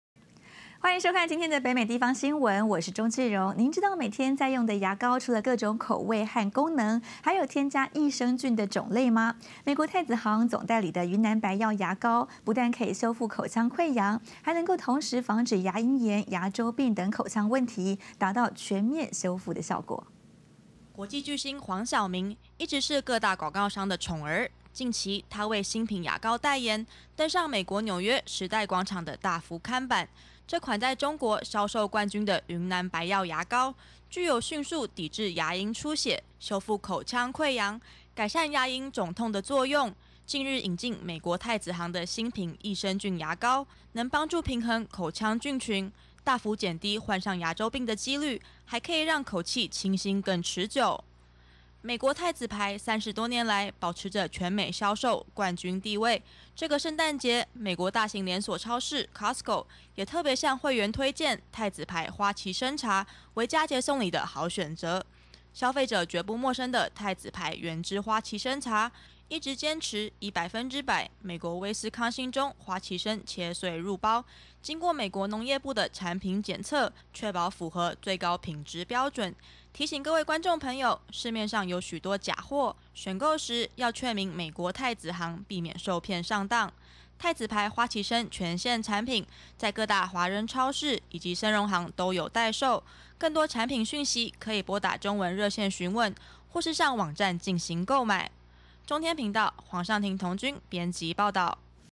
2017.12.20中天北美新聞-1 美國太子行熱銷雲南白藥牙膏 黃曉明強勢代言
您知道每天在用的牙膏，除了各種口味和功能，還有添加益生菌的種類嗎？美國太子行總代理的雲南白藥牙膏，不但可以修復口腔潰瘍，還能同時防止牙齦炎、牙周病等口腔問題，達到全面修復的效果。